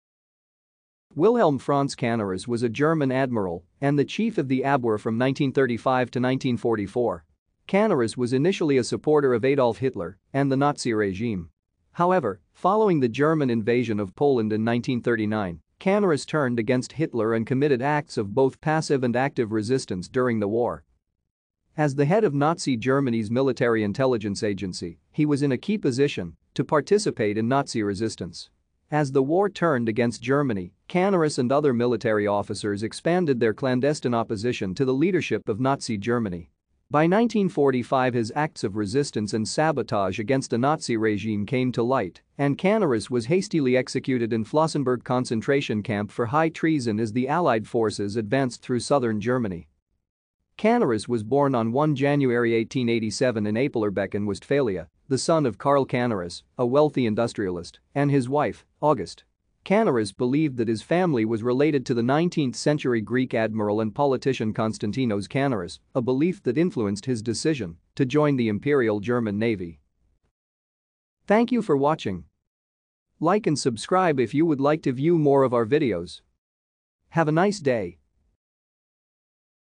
Wilhelm Canaris
Wilhelm Franz Canaris  was a German admiral and the chief of the Abwehr  from 1935 to 1944. Canaris was initially a  supporter of Adolf Hitler, and the Nazi regime. However, following the German invasion of Poland in 1939, Canaris turned against Hitler and committed acts of both passive and active resistance during the war.